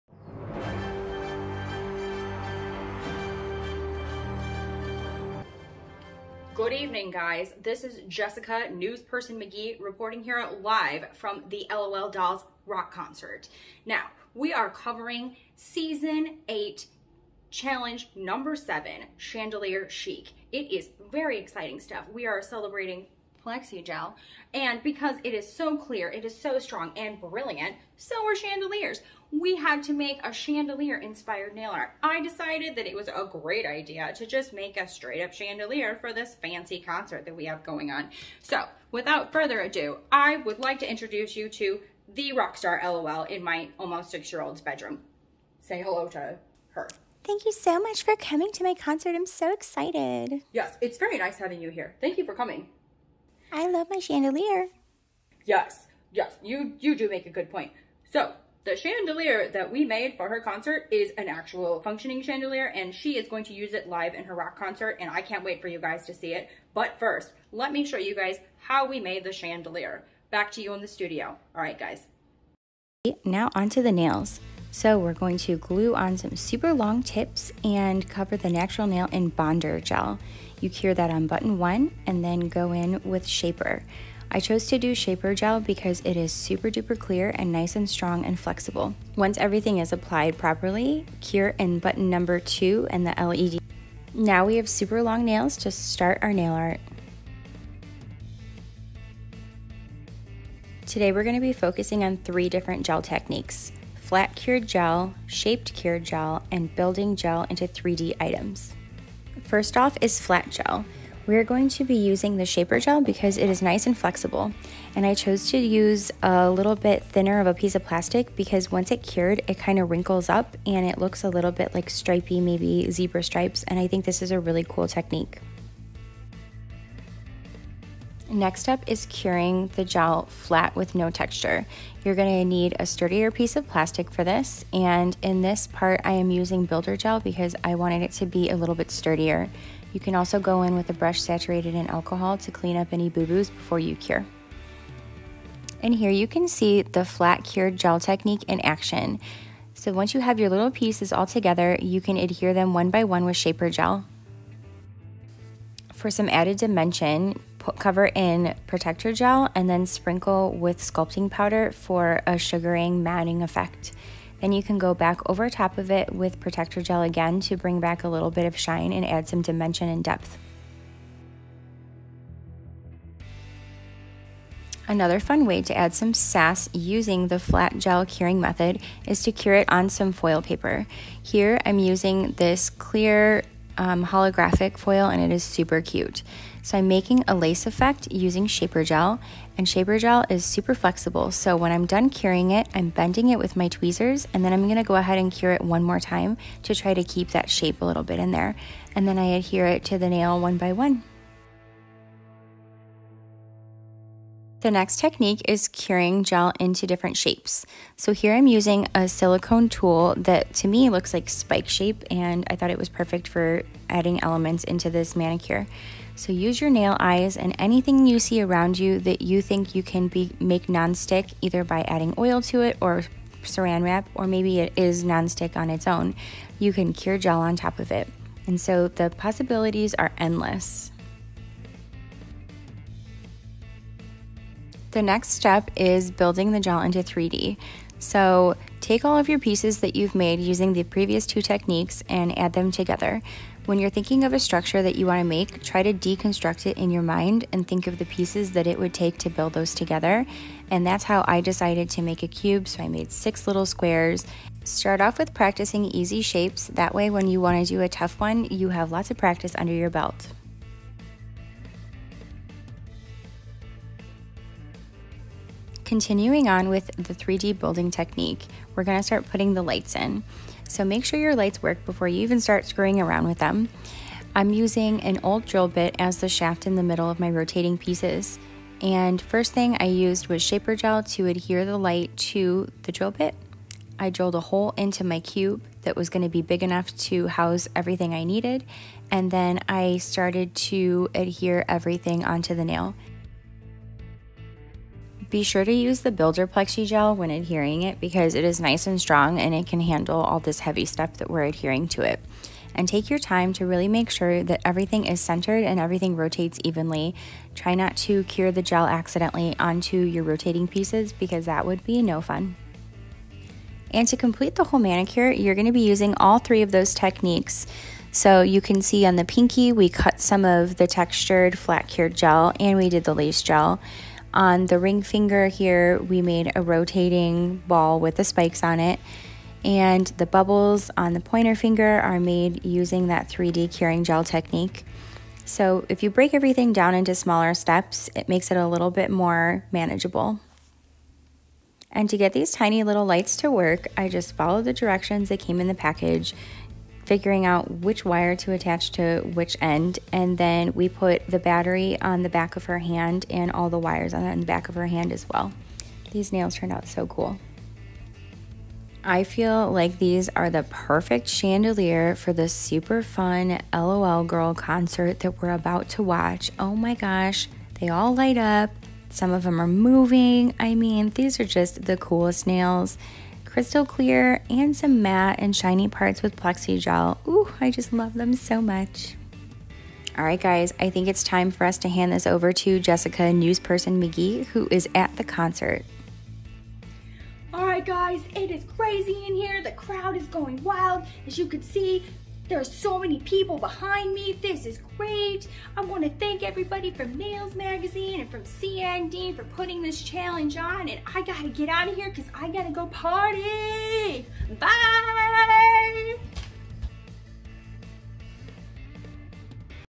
Chandelier Chic
For this challenge, we were tasked with creating chandelier inspired nails to celebrate CND’s Plexigel line! Plexigel is clear, strong, and brilliant so its obvious why chandeliers were this weeks inspiration!
I knew right away that I didnt want to do a traditional chandelier look, so while searching for inspiration photos, I came across a bunch of chandelier art. There were spirals made from hand blown glas, flowers, real rocks and minerals, brilliant AMAZING crystal work, fabric, and even some that look like real clouds!
My daughter, Dolly asked me to make nails for her rock and roll doll so in honor of her birthday this week I did just that!
Her doll reminded me of Lady Gaga so I dove into her costume looks for inspiration! There are spikes, cubes, diamond shaped scales, and bubbles among the design element in this look. To elevate these nails and have more fun in the process I decided to make them light up! So now that cool rock and roll doll has her own nail chandelier for her rock concerts!
Everything in this look was constructed with Plexigel, and I used the help of matte top coat, original Shellac top coat, plus some clear sculpting powder to give it a sugaring texture effect.
I love how these nails bounce light around beautifully, just like a real chandelier does!
Thank you NAILS magazine and CND for this challenge, it really lit me up!!